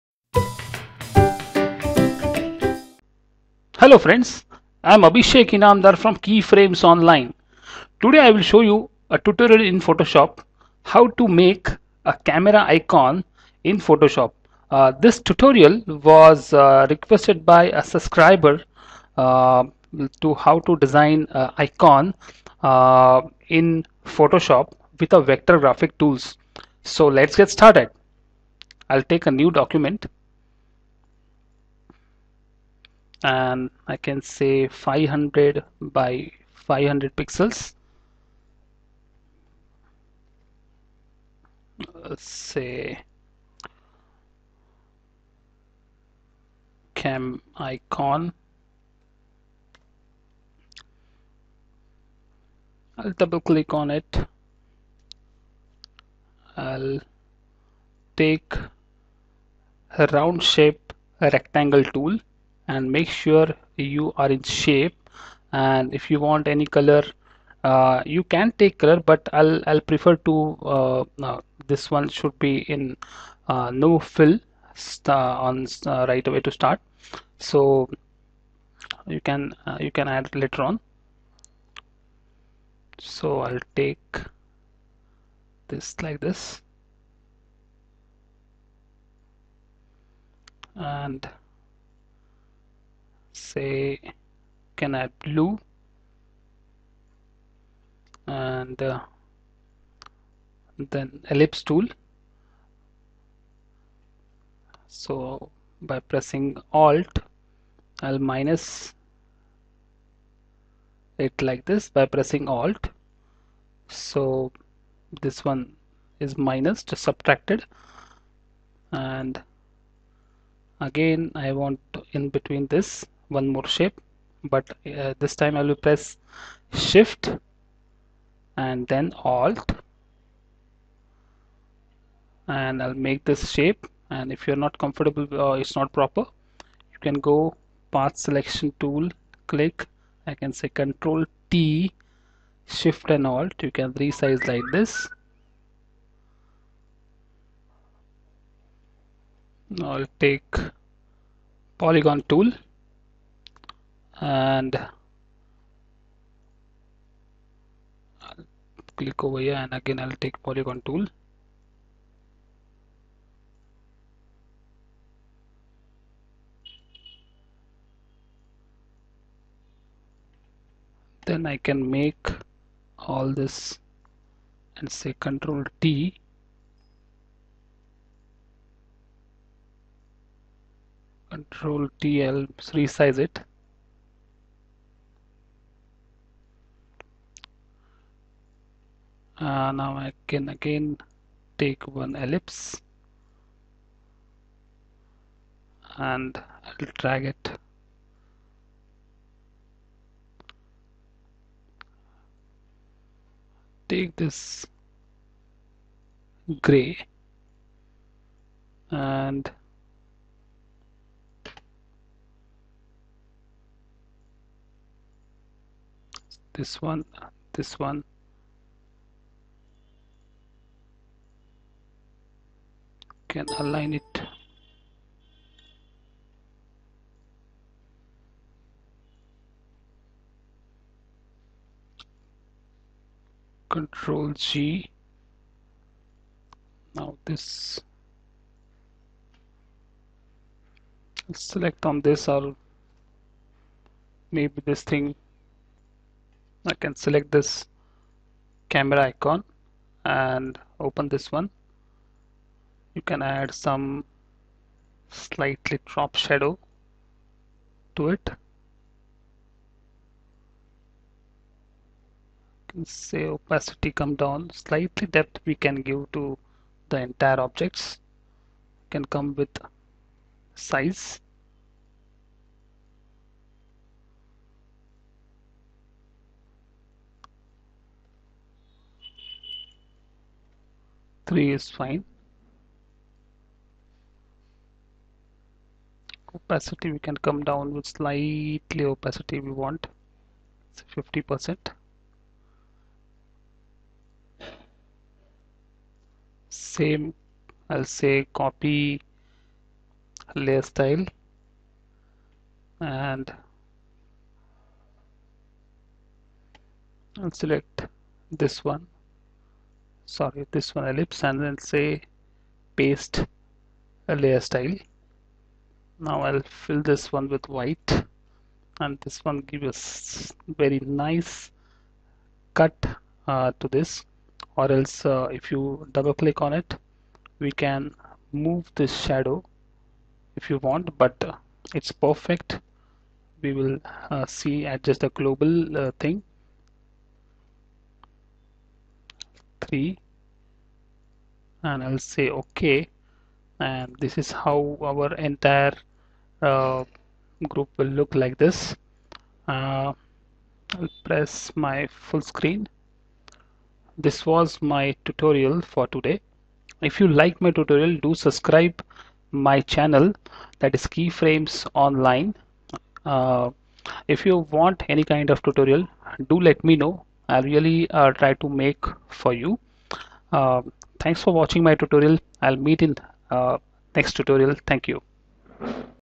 Learn how to make icon in Adobe Photoshop with vector shape
Adobe Photoshop Tutorial:In this tutorial,i have used Vector Shapes
For Making Camera icon,Which can be re-sized
at any extent.
You can take this as example and try your own
shapes as per your need...
Keyframesonline
Enjoy Learning. !